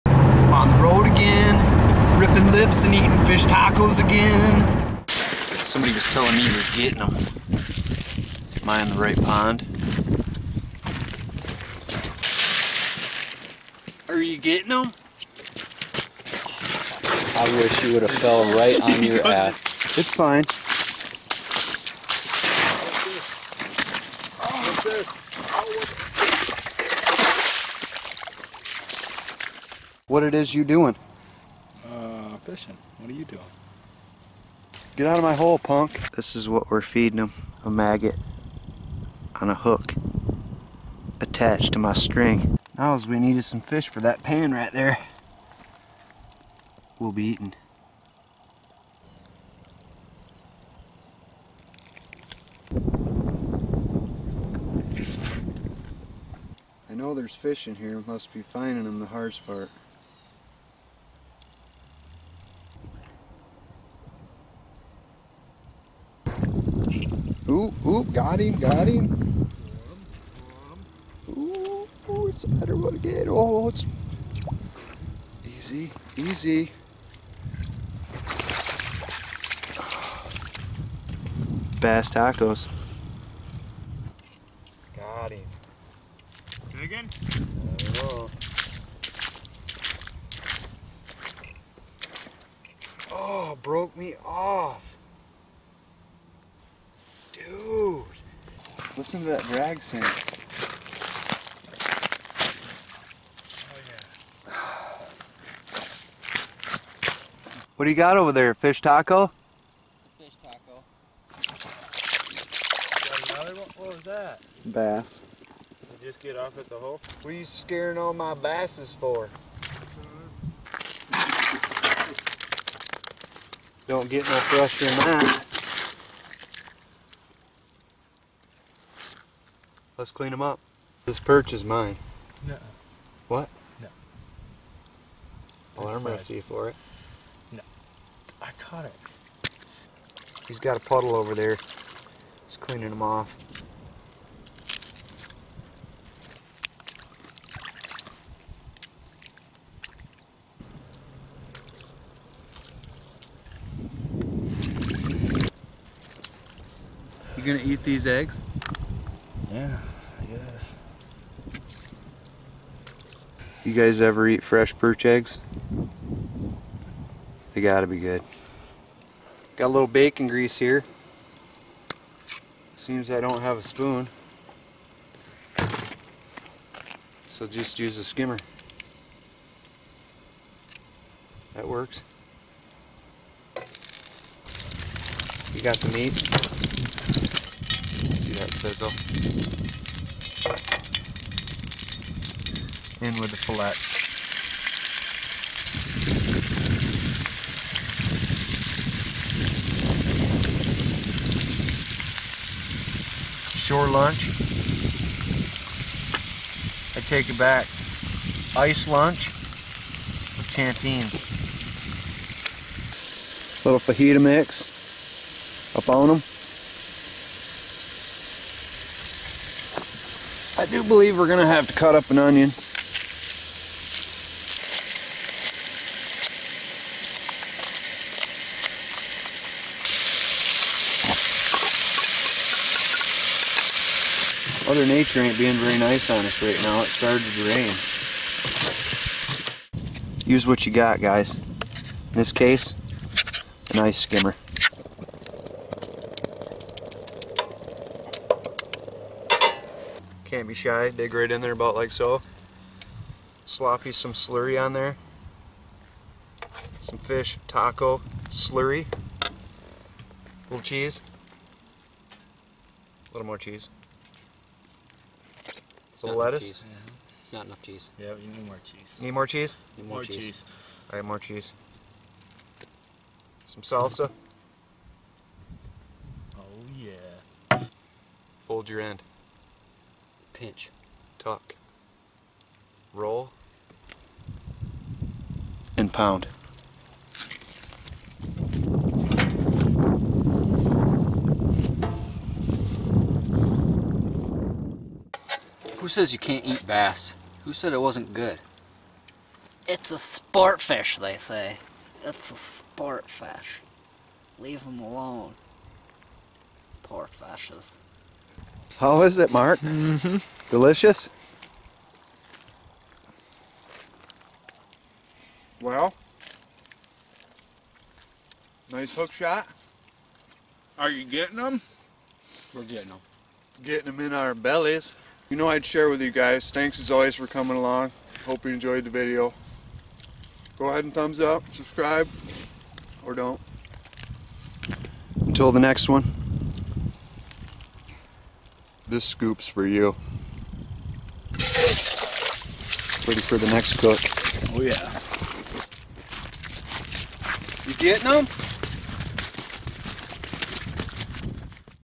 Catch and Cook in the RAIN on the ICE - Delicious Fish Tacos!!
We got permission to fish some new water on private land, the owner stocks perch and gills in. He told us to keep all bass because there over populated and choking out other species. So why not grab a pan and cook some!! DELISH!! Turns out small bass eat mighty fine, clean too!